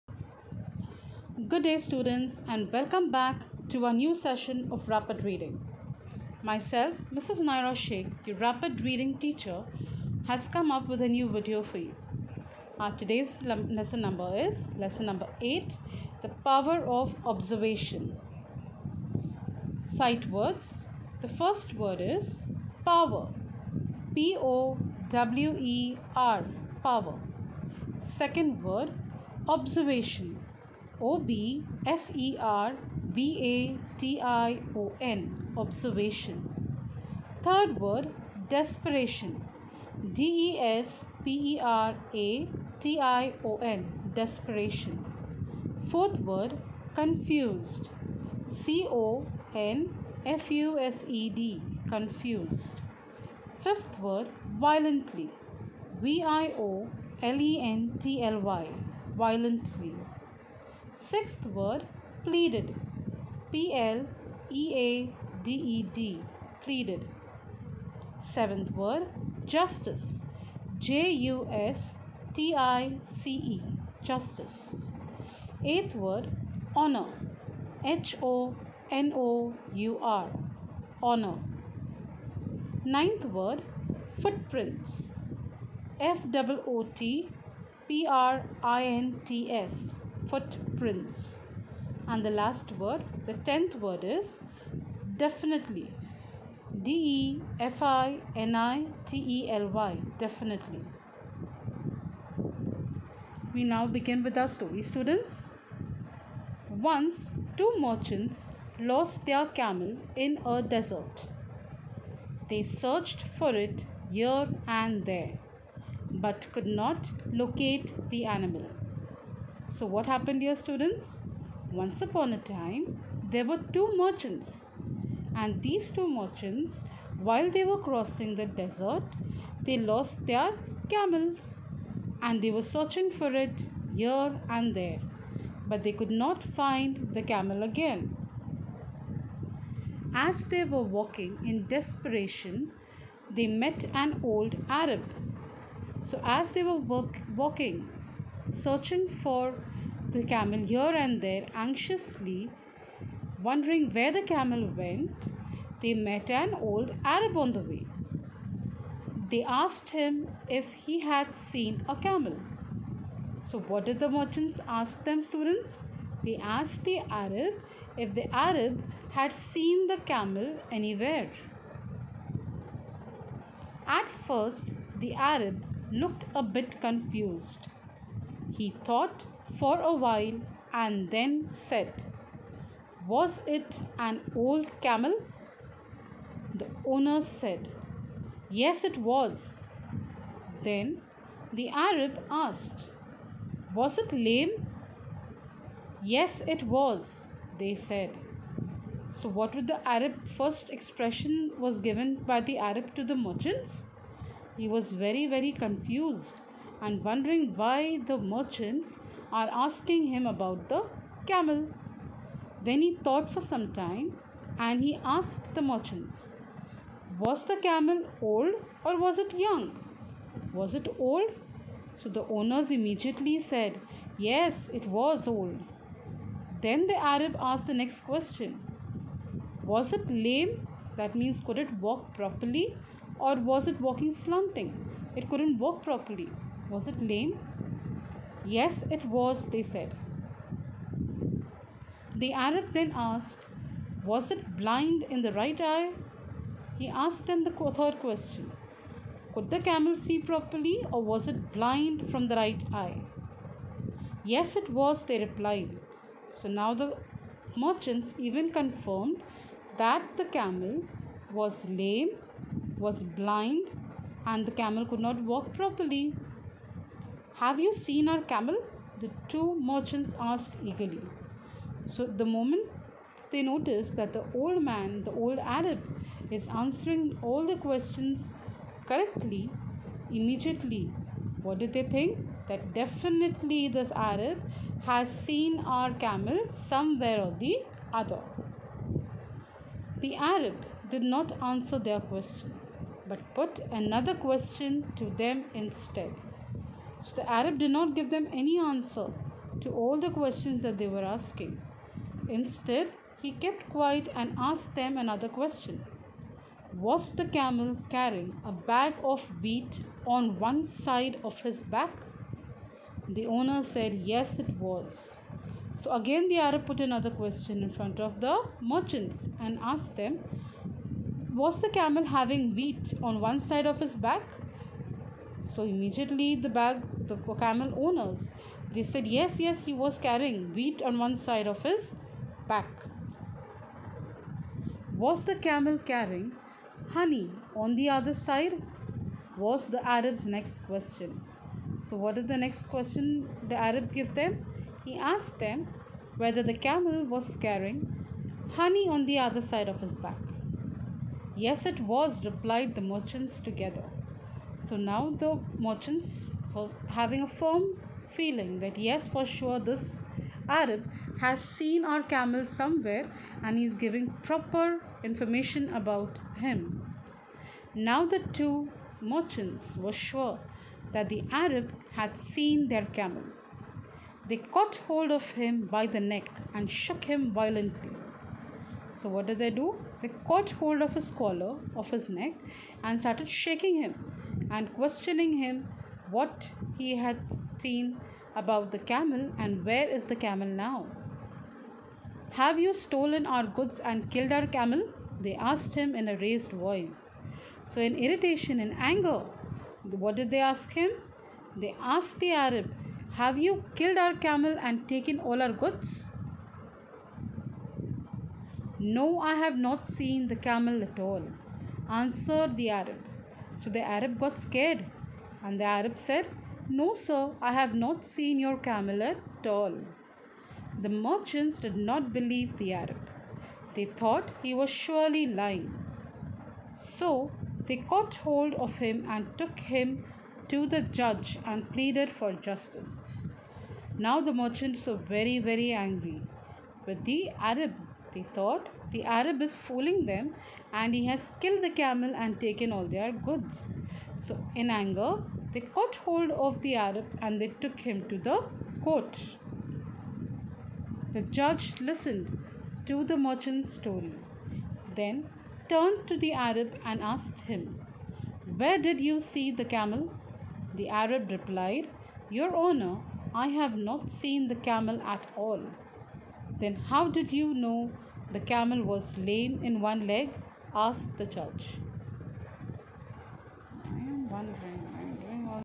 Std -IV. Subject -Rapid Reading. Topic -The Power of Observation
In this video we will learn about how our power of Observation helps us in our day to day life.